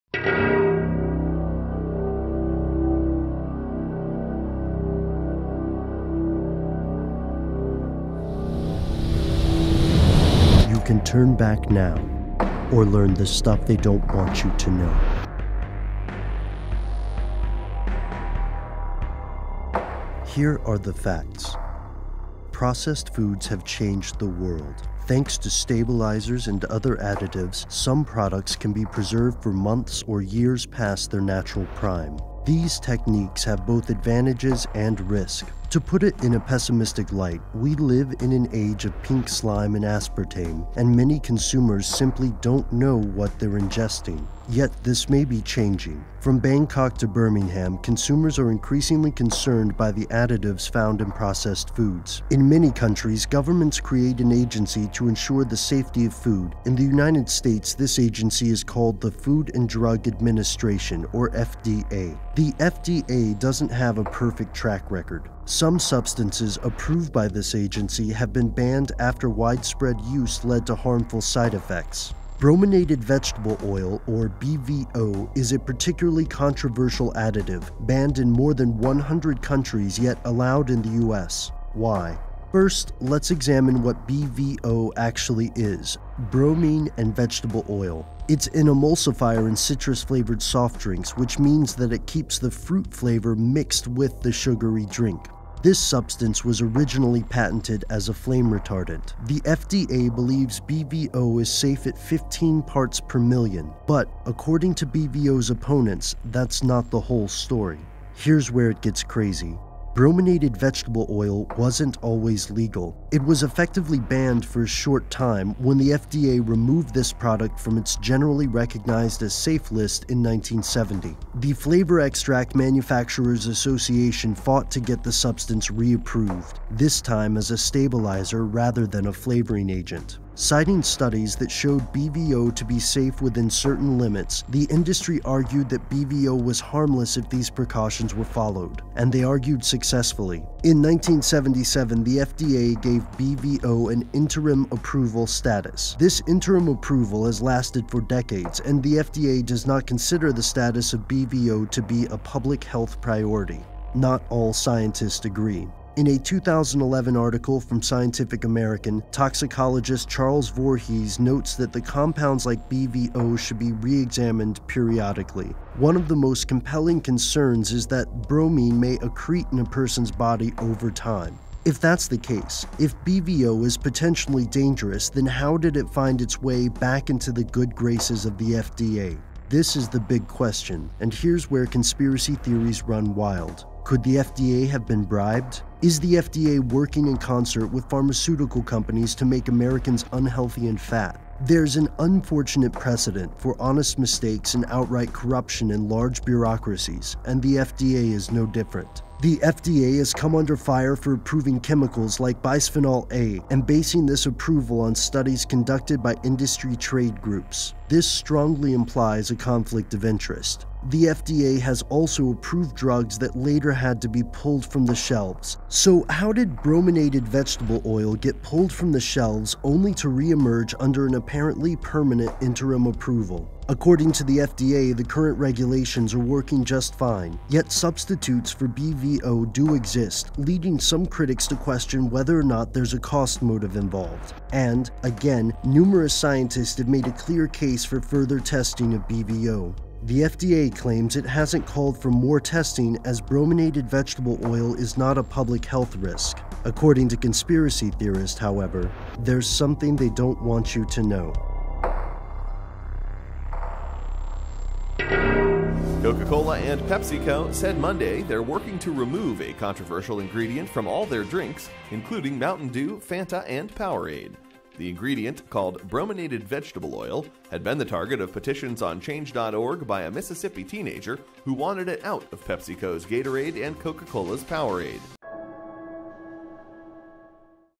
What is brominated vegetable oil? | CLASSIC
Brominated vegetable oil is in a surprisingly large range of food products - but what is it, exactly? Are the health concerns surrounding this ingredient alarmist or is there something [you know the rest]?